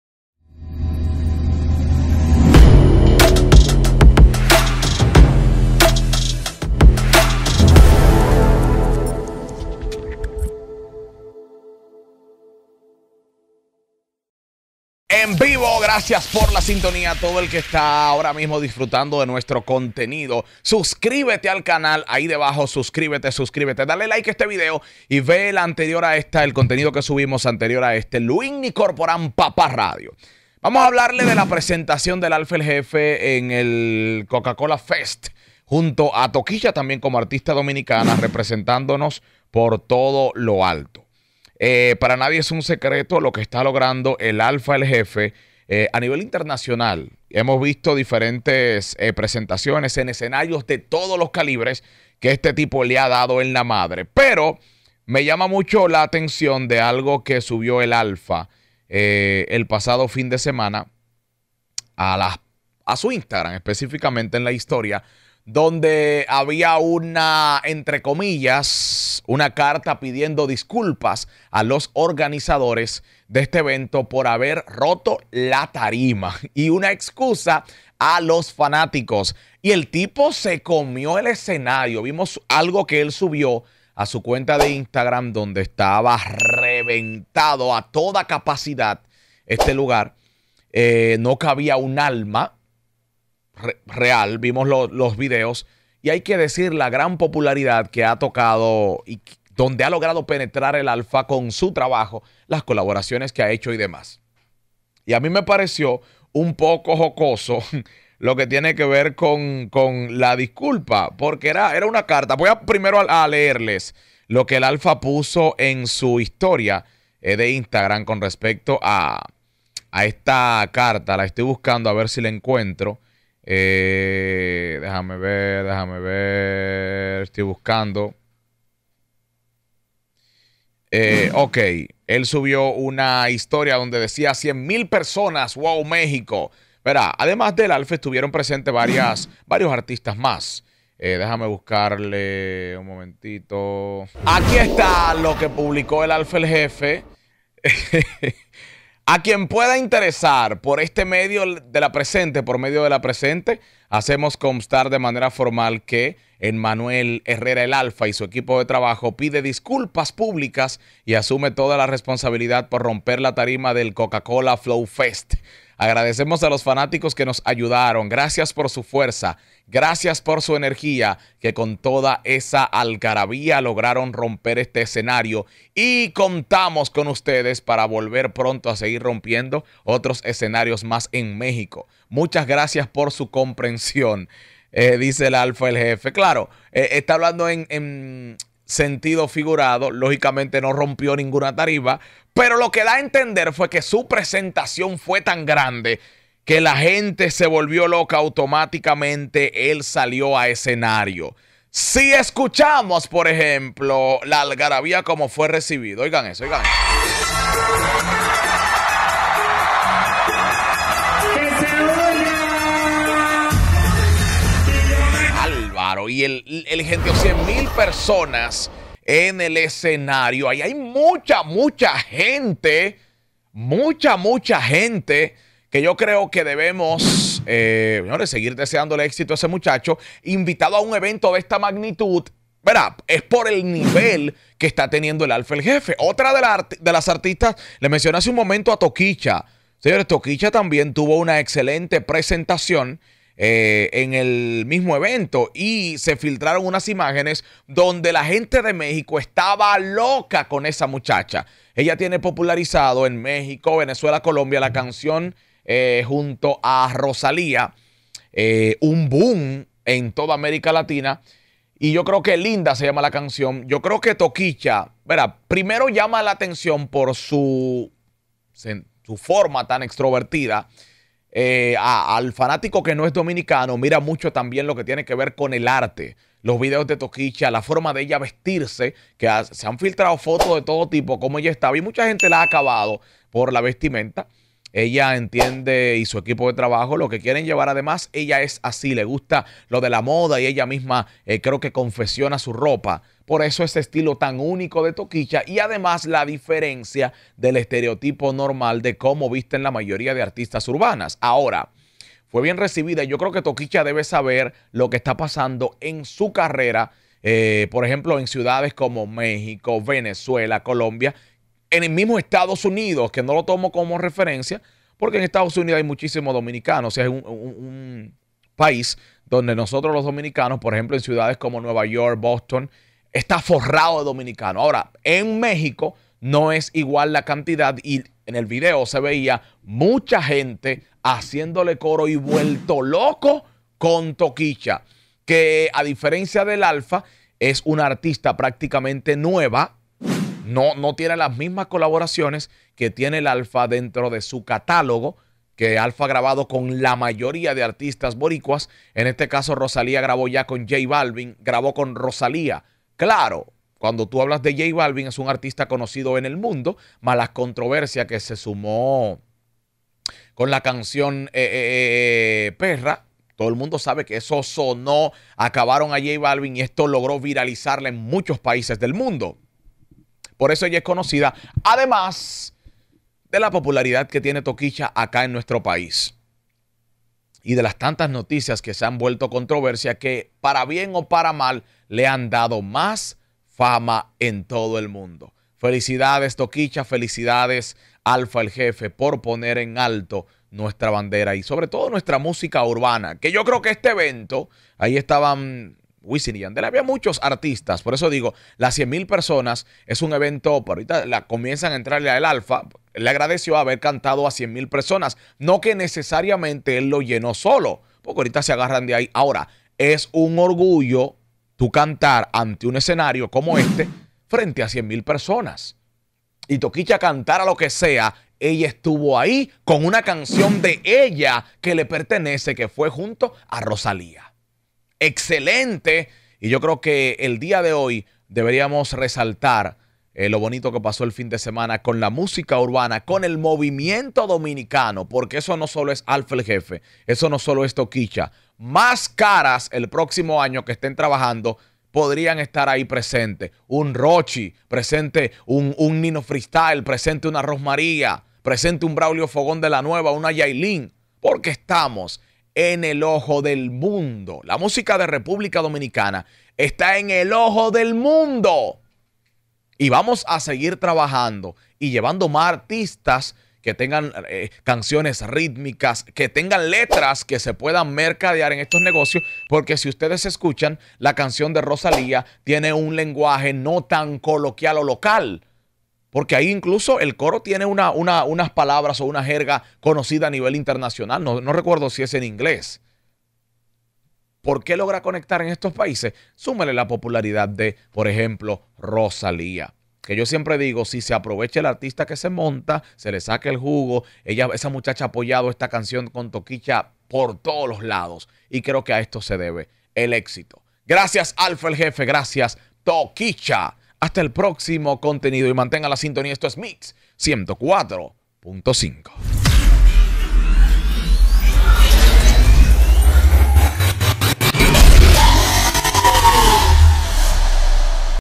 EL ALFA “EL JEFE” LO NUNCA ANTES VISTO EN EL DEMBOW “CANTA A MÁS DE 100 MIL PERSONAS EN MEXICO”
Descarga mi aplicación Luinny Corporan